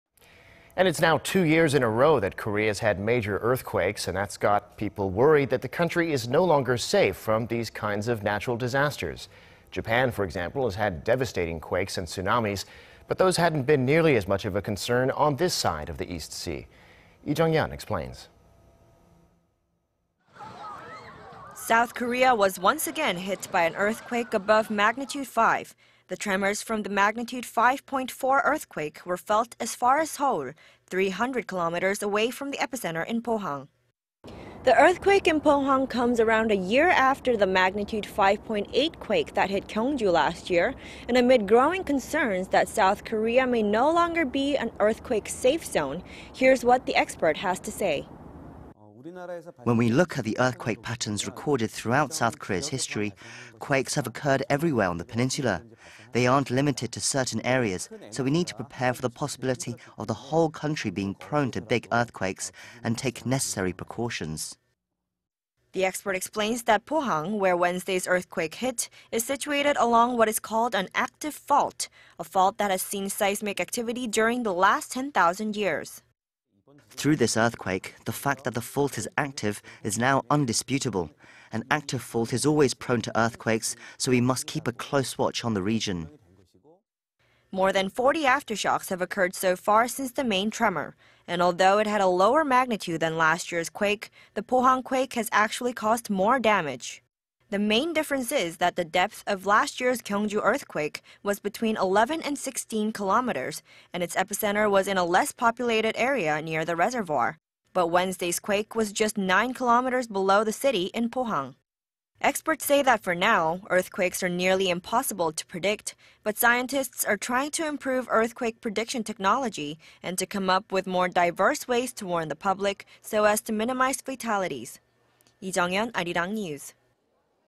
S. Korea no longer 'earthquake-safe' zone... two magnitude 5.0+ quakes in two consecutive years
작년 ‘경주 지진’ 이번엔 '포항 지진'.. 한반도 지진 안전지대 아니다

  It's now two years in a row that Korea's had major earthquakes, and that's got people worried that the country is no longer safe from these kinds of natural disasters.
Japan, of course, has had devastating quakes and tsunamis, but those hadn't been nearly as much of a concern on this side of the East Sea.
Lee Jeong-yeon explains.
South Korea was once again hit by an earthquake above magnitude five.
  The tremors from the magnitude 5-point-4 earthquake were felt as far as Seoul, 300-kilometers away from the epicenter in Pohang.

(stand-up)
   The earthquake in Pohang comes around a year after the magnitude 5-point-8 earthquake that hit Gyeongju last year. So amid growing concerns among the public that South Korea may not be an "earthquake-safe" zone, here is what the expert has to say.

(Korean)
   "When we look at the earthquake patterns recorded throughout South Korea's history, quakes have occurred everywhere on the peninsula. They aren't limited to certain areas. So we need to prepare for the possibility of the whole country being prone to big earthquakes and take necessary precautions."

  The expert explains that Pohang, where Wednesday's earthquake hit, is situated along what is called an "active fault," a fault that has seen seismic activity during the last 10,000 years.

(Korean)
   "Through this earthquake, the fact that the fault is active is now undisputable. An active fault is always prone to earthquakes so we must keep a close watch on the region."

 More than 40 aftershocks have occurred so far since the main tremor. And although it had a lower magnitude than last year's quake, the Pohang quake has actually caused more damage.
 The main difference is that the depth of the of last year's Gyeongju earthquake was between 11 and 16 kilometers, and its epicenter was in a less populated area near the reservoir, but Wednesday's quake was just nine kilometers below the city in Pohang.

 Experts say that for now, earthquakes are nearly impossible to predict, but scientists are trying to improve earthquake prediction technology and to come up with more diverse ways to warn the public... so as to minimize fatalities.

Lee Jeong-yeon, Arirang News